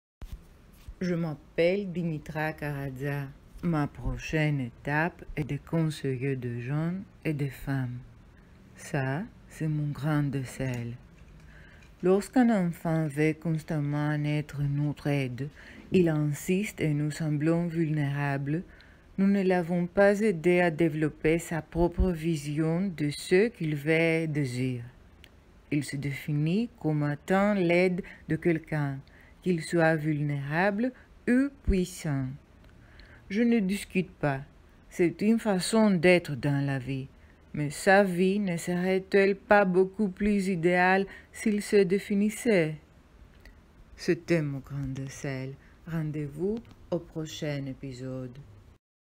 Mon grain de s-elle/ Les enfants qui deviennent les aides des adultes/ Épisode 11
Les enfants qui deviennent les aides des adultes.